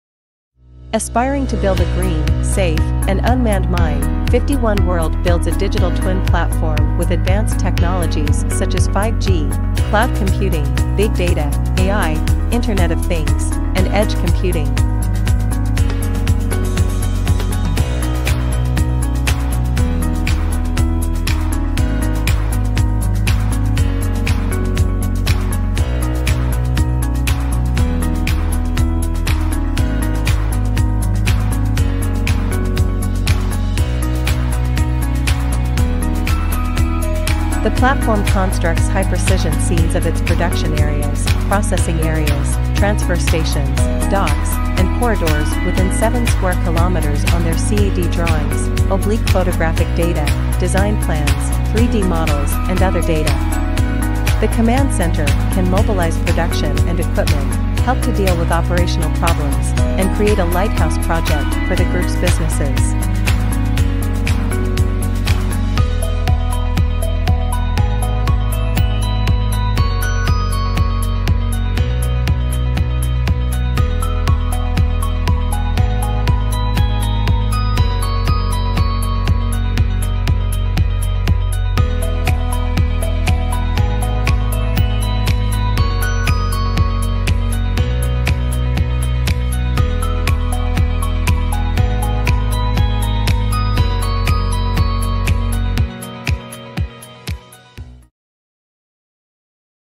Digital Twin Smart Mining-Powered by 51WORLD
Aspired to build a green, safe, and unmanned mine, 51WORLD built a digital twin platform for the client with advanced technologies such as 5G, cloud computing, big data, AI, Internet of Things, and edge computing.
The platform constructs high-precision scenes of its production areas, processing areas, transfer stations, docks, and corridors within 7 square kilometers on their CAD drawings, oblique photographic data, design plans, 3D models and other data. The command center can mobilize production and equipment, help to deal with operational problems, and create a lighthouse project for the group's businesses.

Follow, Share and Like 51WORLD to learn more about the leading digital twin, metaverse and simulation technologies